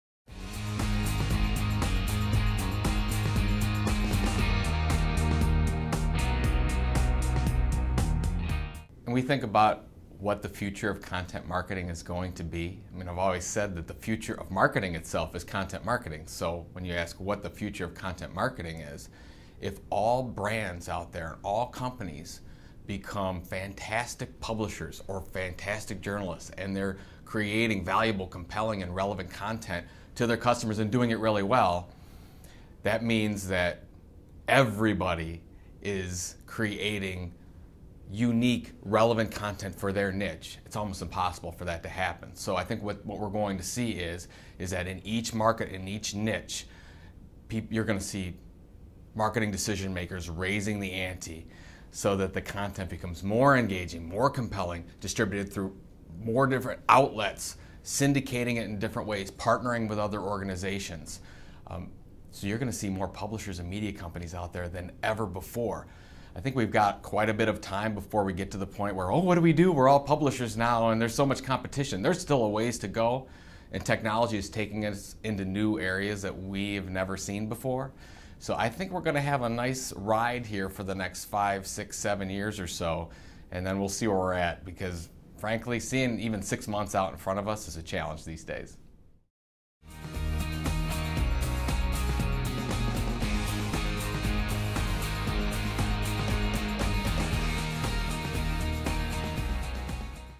Content Marketing: What is the Future of Content Marketing?
If the future of marketing is content marketing, than what will the future of content marketing look like? Joe Pulizzi, Founder of Junta 42 and the Content Marketing Institute, talks about how publishers and media companies will likely up the ante on content creation going forward.